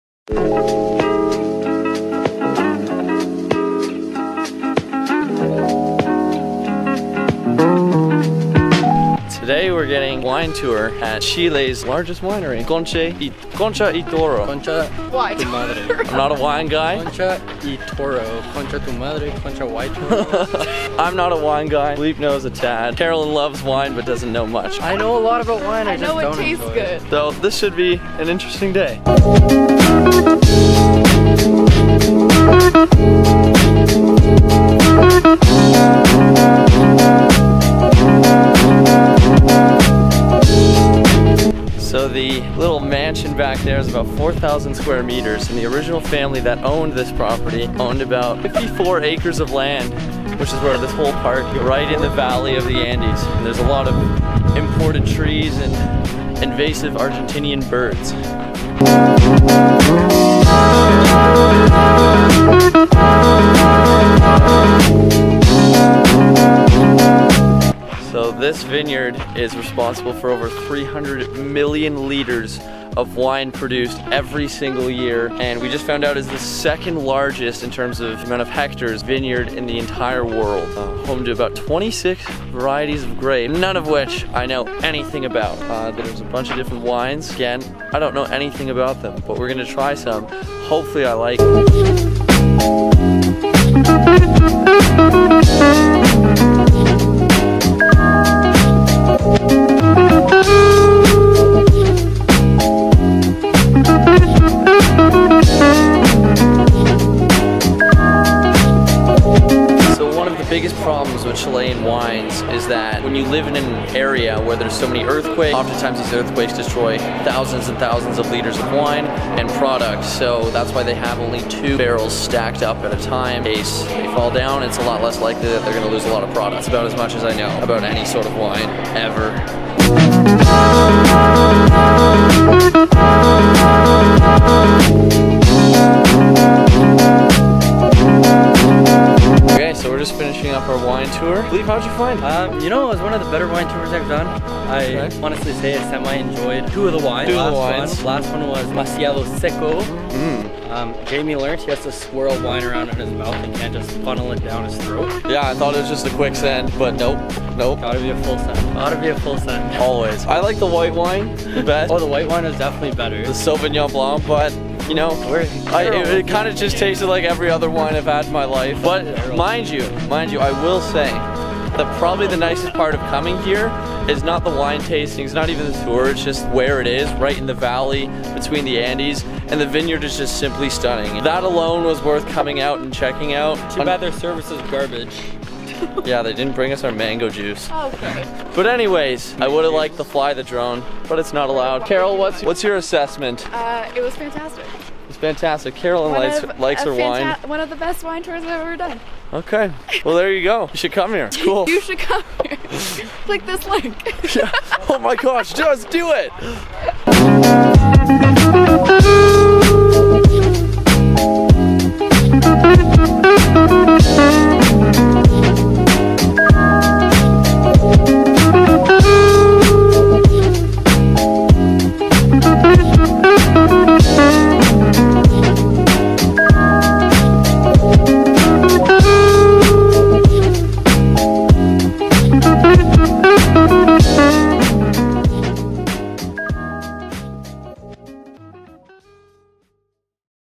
LARGEST VINEYARD IN SOUTH AMERICA - Concha Y Toro, Chile
We had the opportunity to visit the second largest winery in the world and increase our palettes. None of us really know anything about wine so it was neat to learn about the process, and even cooler to see the valley where the grapes are grown.

The cheapest 4K camera ever